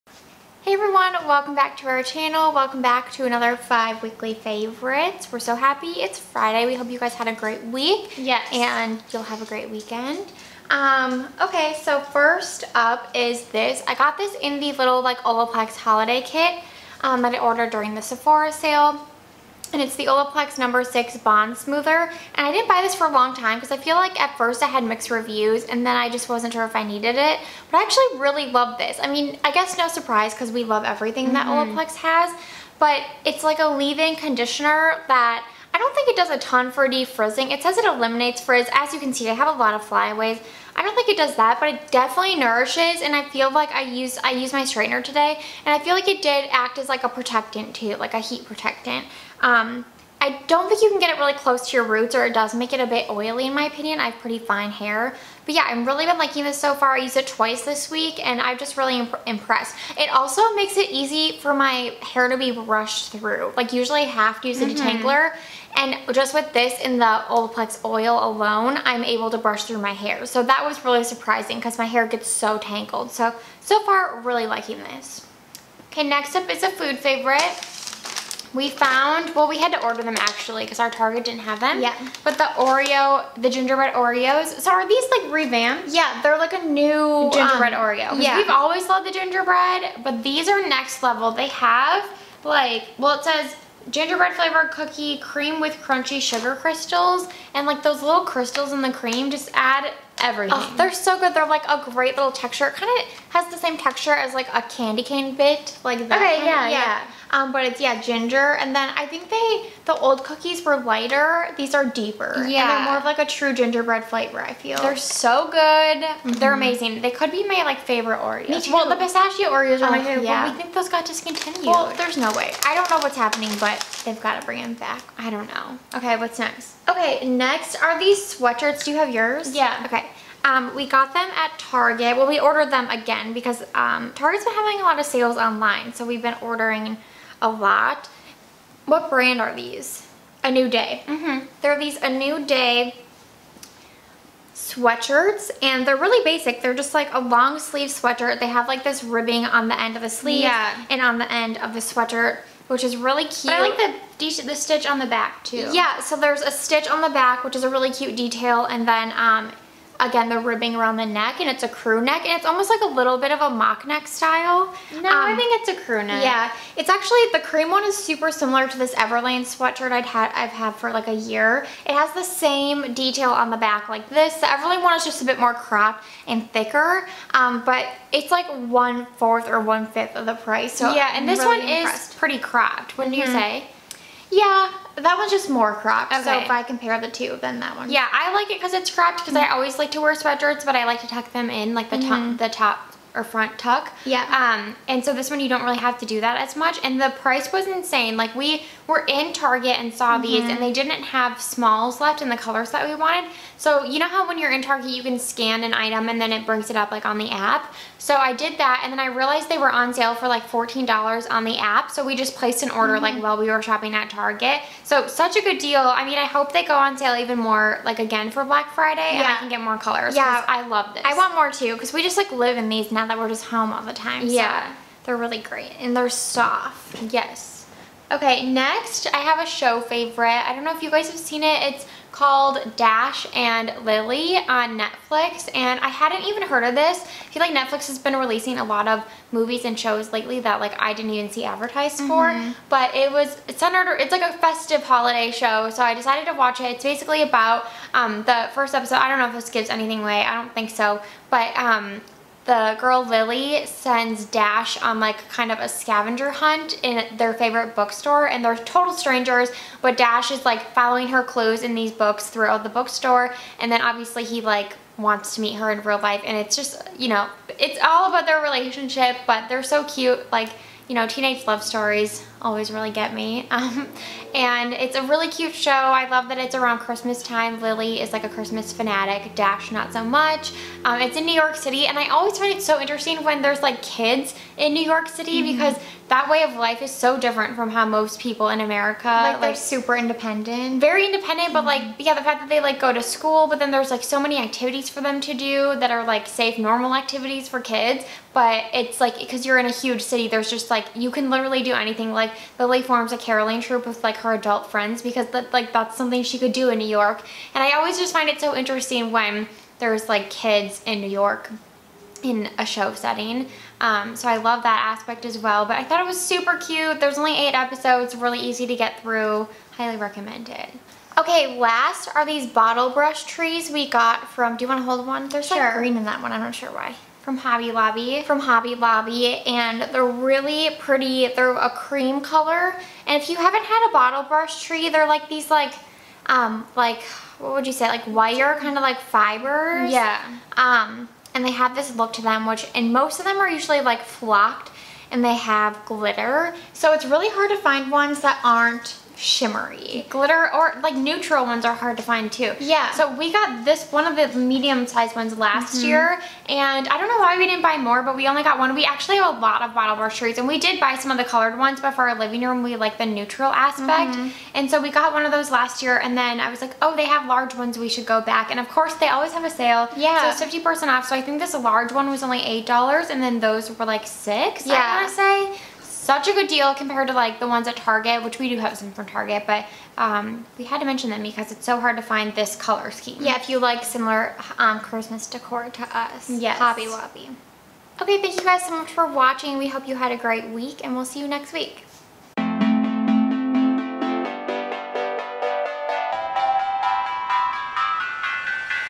Five Weekly Favorites | Week 120
Watch to see what we've been loving this week! xx, B+C

• Olaplex No.6 Bond Smoother:
• Gingerbread OREOS:
• A New Day Sweatshirts:
• Dash & Lily on Netflix
• Bottle Brush Trees:

Amazon favorites